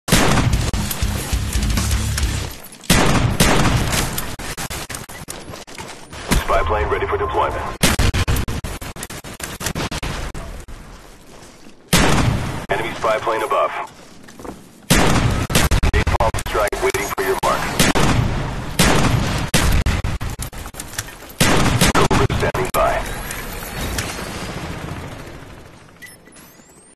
Super Quick 7 Killstreak
I absolutely wreck 7 people with the Olympia shotgun in less than 30 seconds, including 3 dummies who keep coming up the stairs.